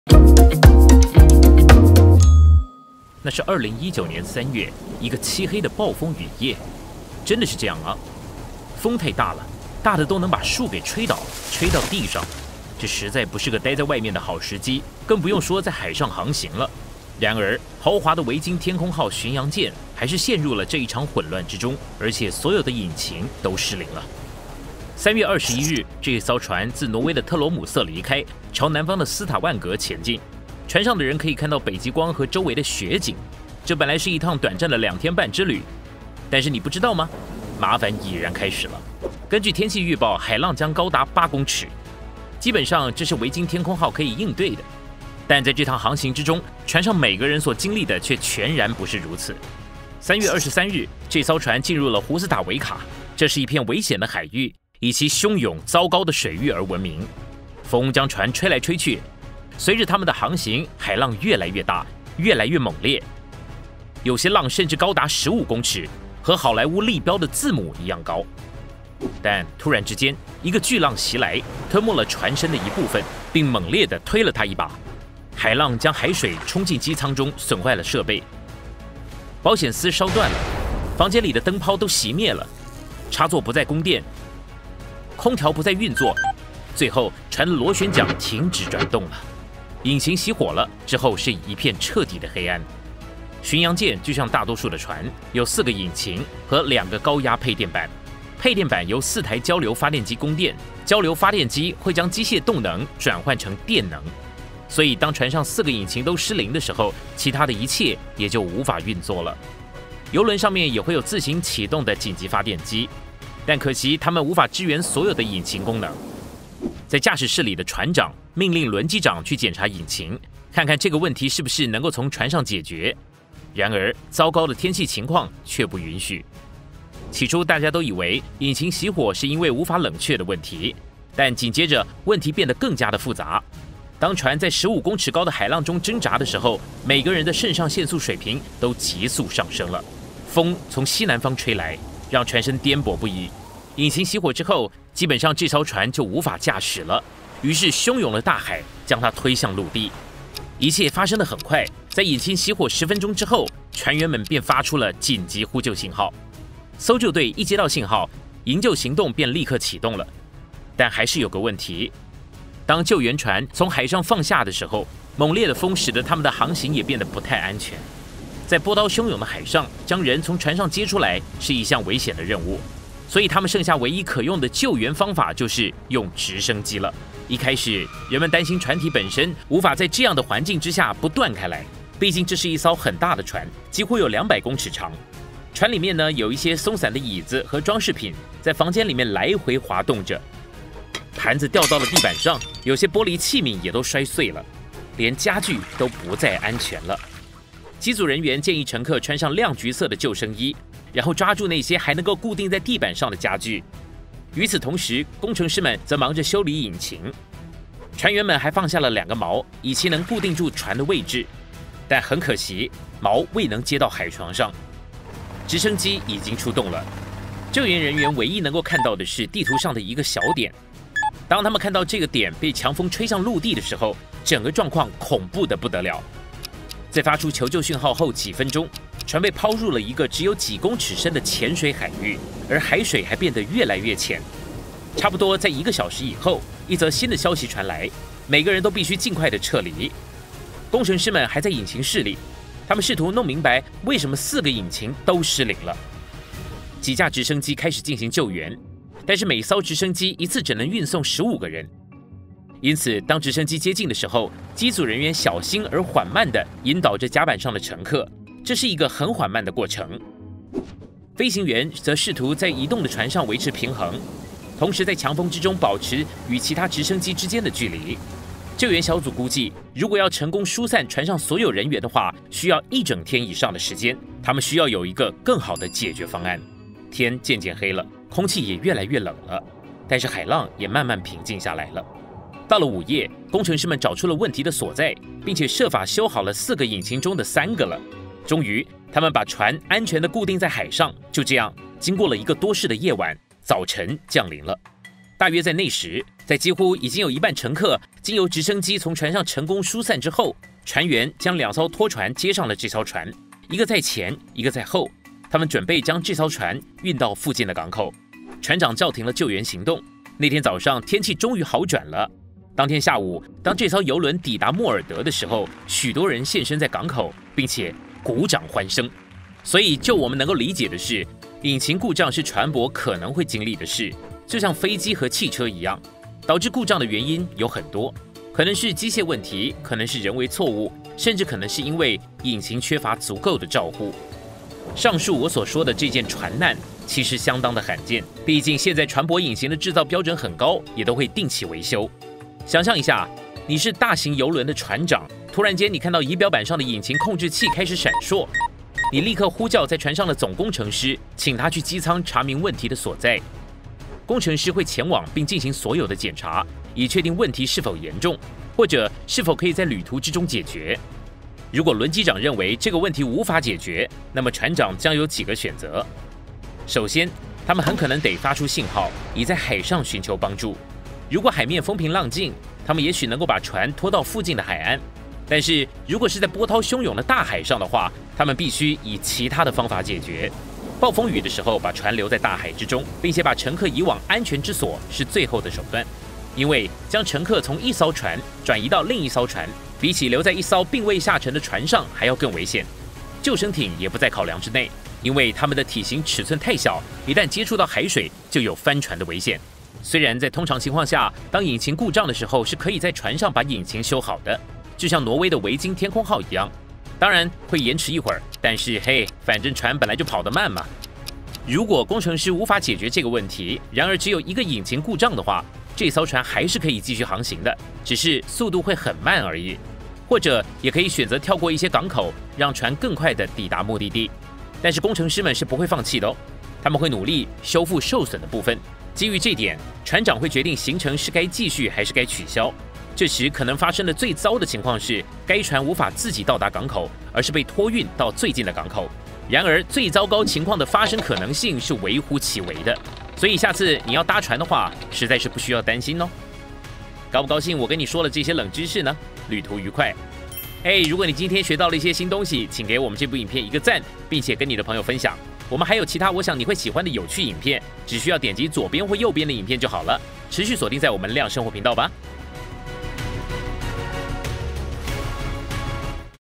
如果船的引擎在海上失靈該怎麼辦
如果船的引擎在海上失靈該怎麼辦? 會發生什麼事呢? 如果引擎失靈了，船長會採取什麼動作呢? 一起來看看吧!那是2019年3月一個漆黑的暴風雨夜。真的是這樣哦…!風太大了，大得都能把樹給吹倒，吹到地上。這實在不是個待在外面的好時機，更不用說在海上航行了。然而，豪華的維京天空號巡洋艦還是陷入了這一場混亂之中，且所有的引擎都失靈了。

3月21日，這艘船自挪威的特羅姆瑟離開，朝南方的斯塔萬格前進。船上的人可以看到北極光和周圍的雪景。這本來是一趟短暫的兩天半之旅，但是麻煩已然開始了。根據天氣預報，海浪將高達8公尺。基本上這是維京天空號可以應對的，但在這趟航行之中，船上每個人所經歷的卻全然不是如此.

123 GO! Chinese

123 GO! Challenge Chinese

123 GO! Gold Chinese